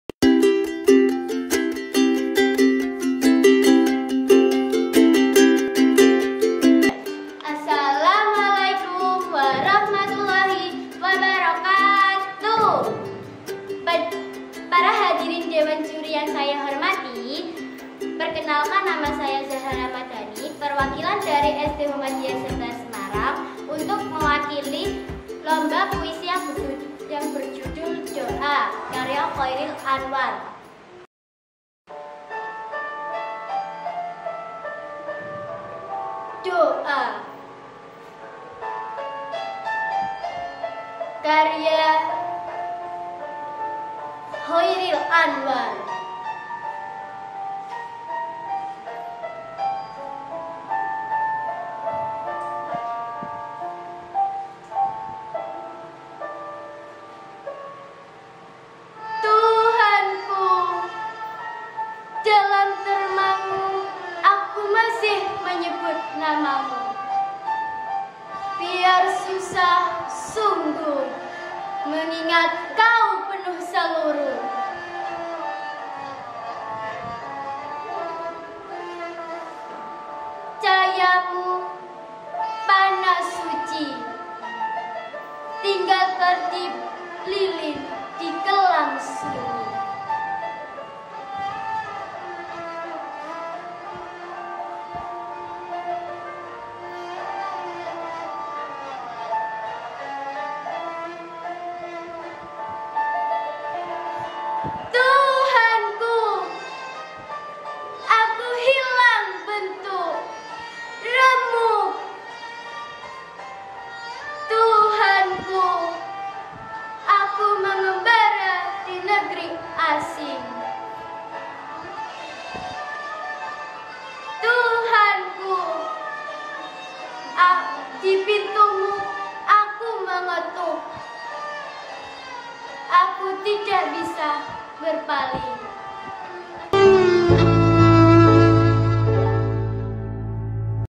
Puisi "DOA" karya Chairil Anwar
Zahra Ramadani siswi kelas 5 SD Muhammadiyah 11 membacakan puisi "Doa" karya Chairil Anwar dalam rangka mengikuti Lomba Baca Puisi